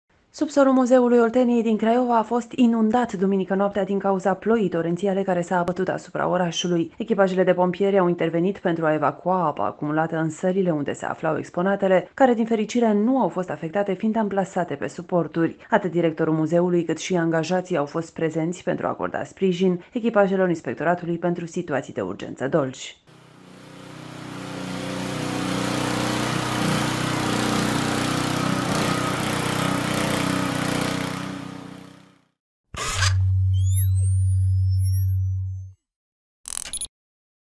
Subsolul Muzeului Olteniei inundat
Subsolul Muzeului Olteniei din Craiova a fost inundat, duminică noaptea, din cauza ploii torențiale care s-a abătut asupra orașului.
Echipajele de pompieri au intervenit pentru a evacua apa acumulata în sălile unde se aflau exponatele care, din fericire, nu au fost afectate fiind amplasate pe suporturi.
Atât directorului muzeului, cat și angajații au fost prezenți pentru a acorda sprijin echipajelor Inspectoratului pentru Situații de Urgență Dolj.